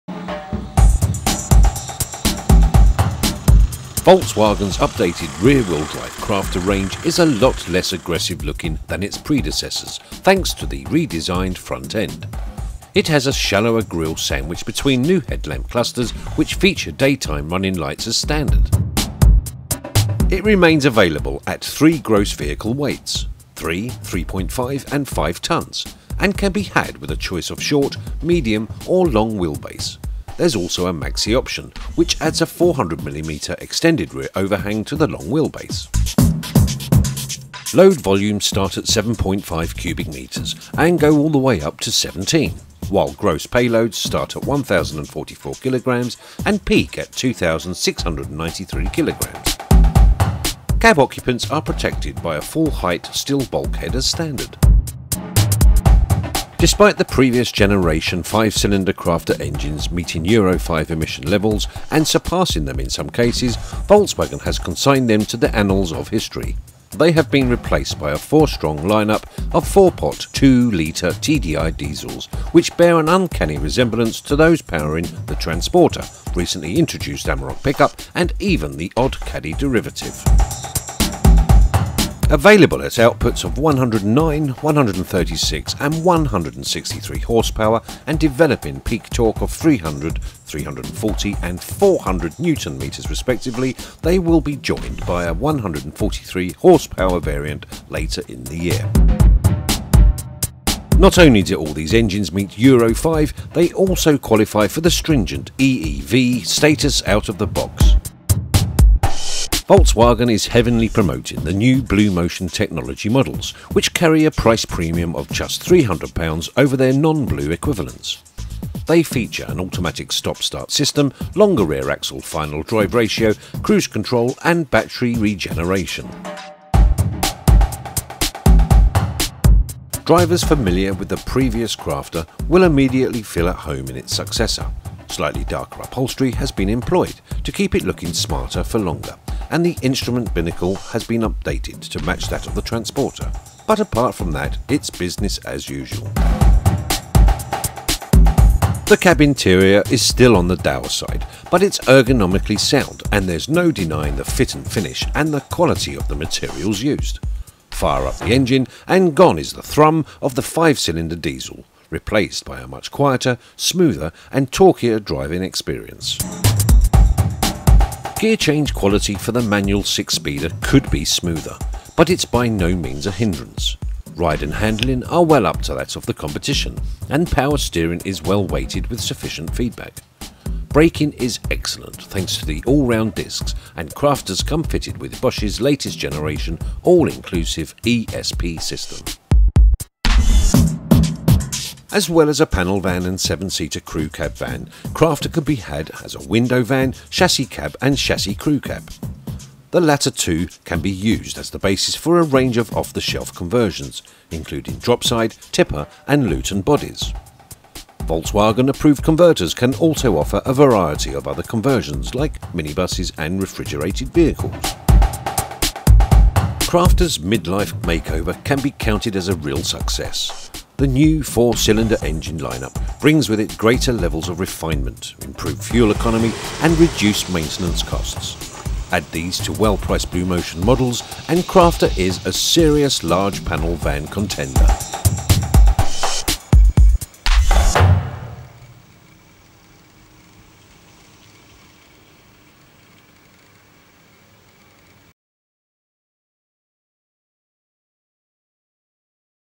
Volkswagen Crafter 2011 UK Launch
Volkswagen's large panel van contender has undergone a serious makeover with changes to the front-end styling and the cab interior, as well as the introduction of a whole new range of four-cylinder Euro 5 diesels.